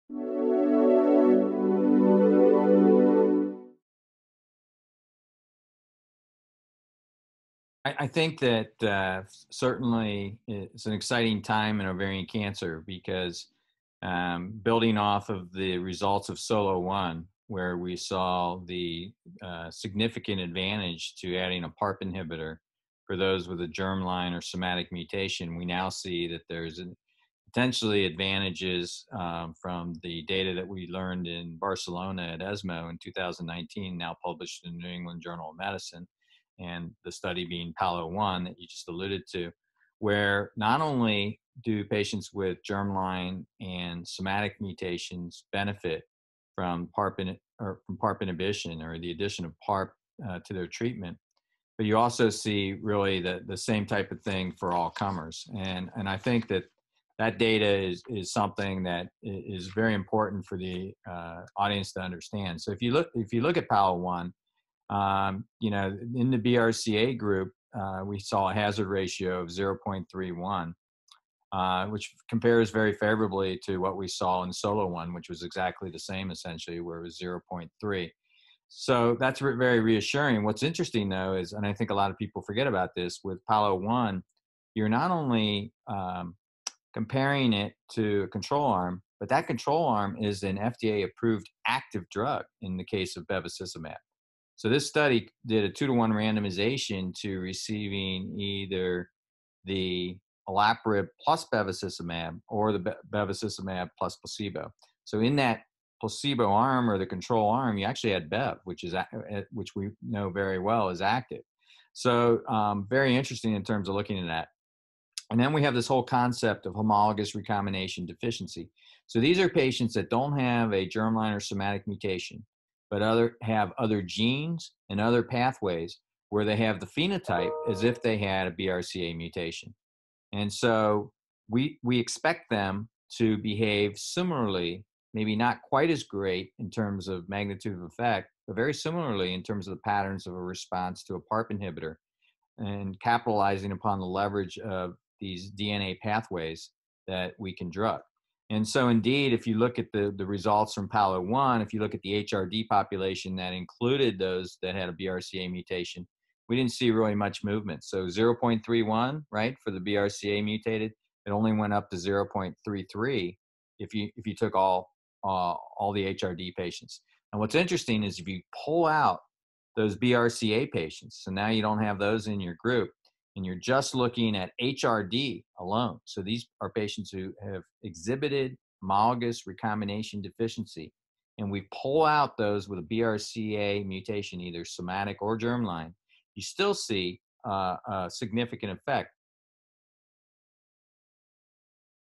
Dr. Herzog on the FDA Approval of Olaparib/Bevacizumab as Frontline Maintenance in Ovarian Cancer
Thomas Herzog, MD, a professor of obstetrics and gynecology and deputy director at University of Cincinnati Cancer Institute, discusses the data that led to the FDA approval of the combination of olaparib and bevacizumab for the maintenance treatment of patients with advanced ovarian cancer who are in complete or partial response to first-line platinum-based chemotherapy with bevacizumab and whose cancer is associated with homologous recombination deficiency–positive status defined by either a deleterious or suspected deleterious BRCA mutation, and/or genomic instability.